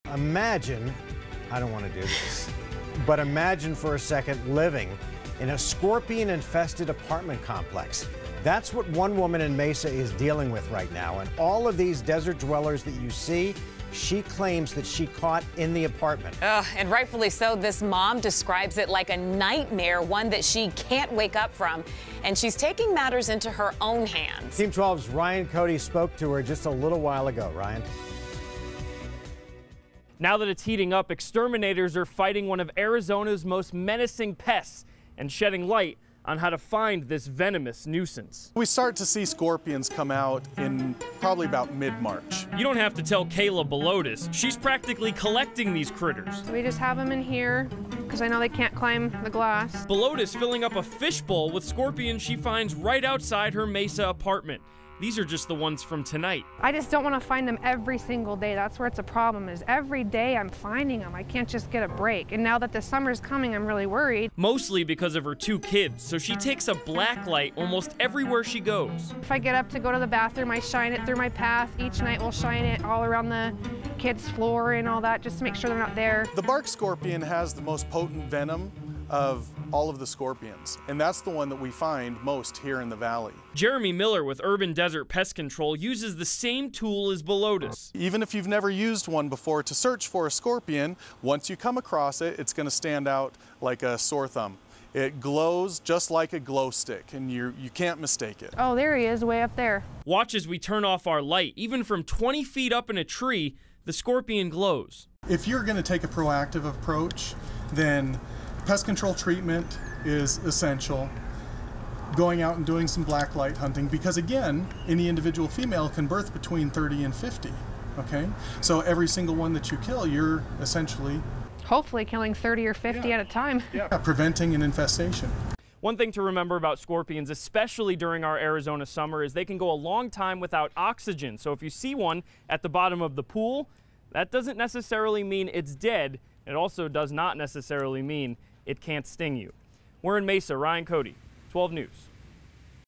Mesa mom says her apartment is infested with scorpions
A Mesa mom has been collecting scorpions she says have been infesting her apartment.